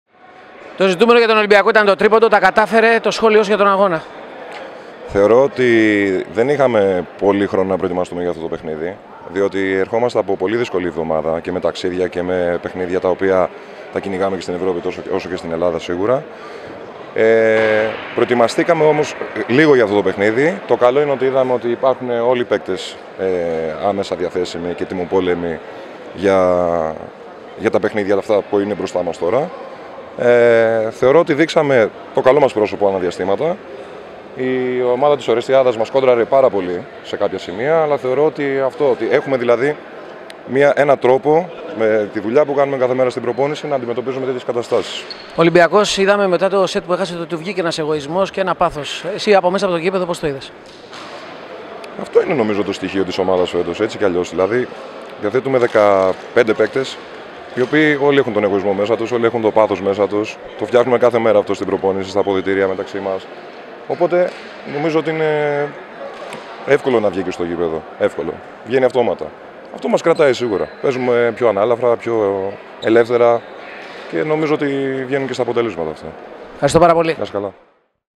Νικόλας Παπαγγελόπουλος : "Έχουμε πάθος και εγωισμό, το φτιάχνουμε καθημερινά στην προπόνηση"
🔴⚪️ Follow us ⤵️